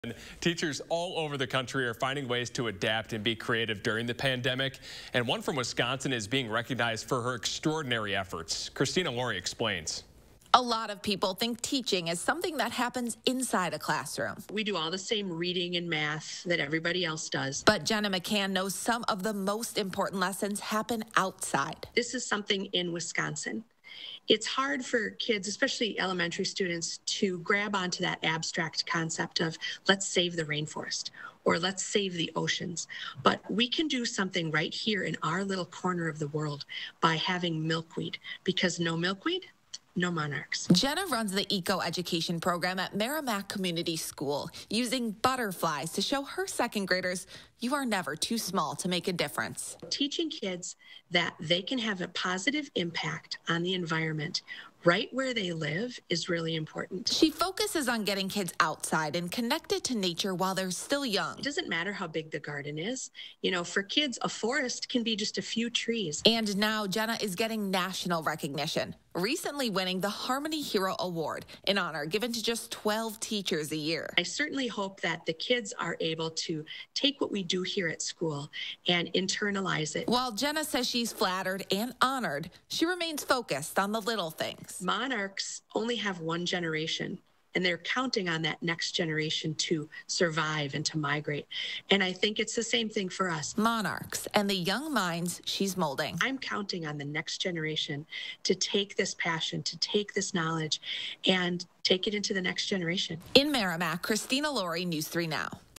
Sauk Prairie teacher wins national award for eco-education program
Jenna McCann is one of only 12 teachers to receive the 'Harmony Hero' award